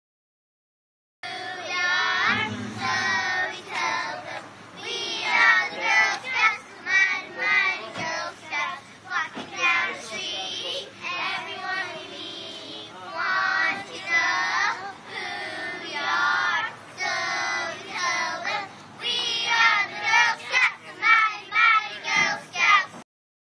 Girl scouts
The girls of Silver Springs performs a cookie chant